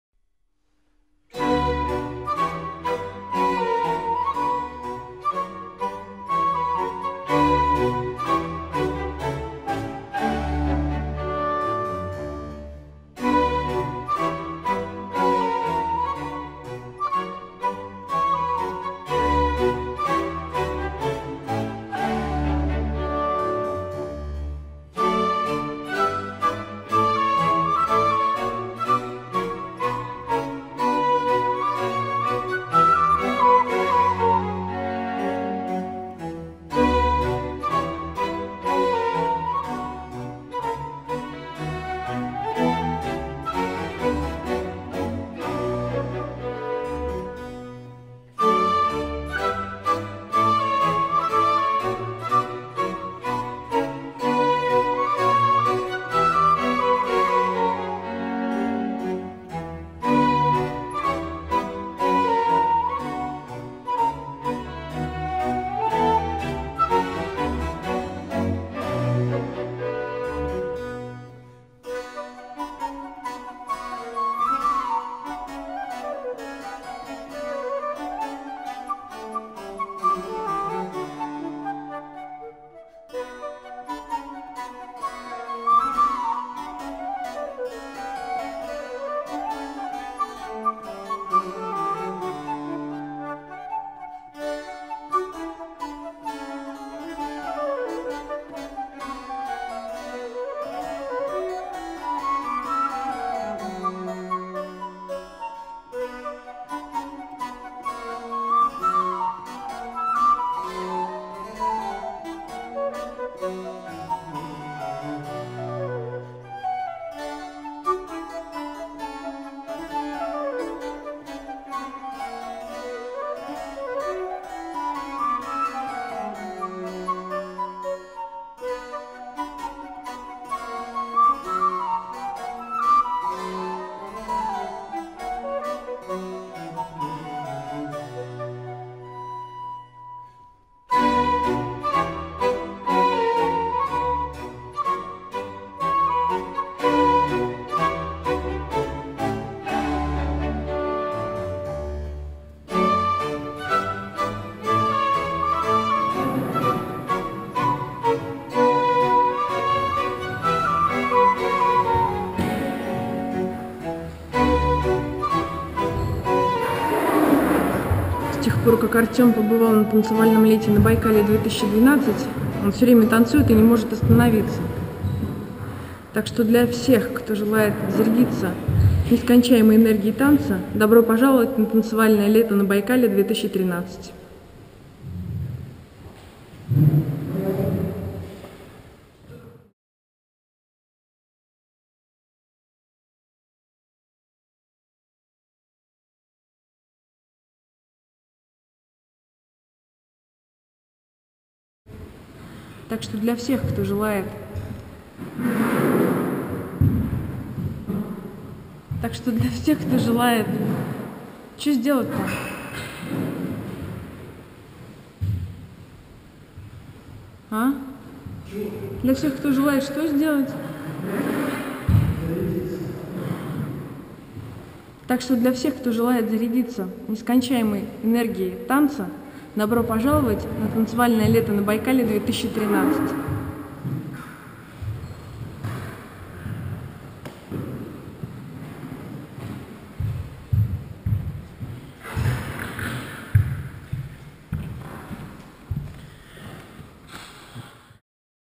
Артем и Настя про "Танцевальное лето на Байкале".
Видео Артема Игнатьева и Анастасии Кадрулевой / DVA.
Педагоги "Танцевального лета на Байкале" 2012г.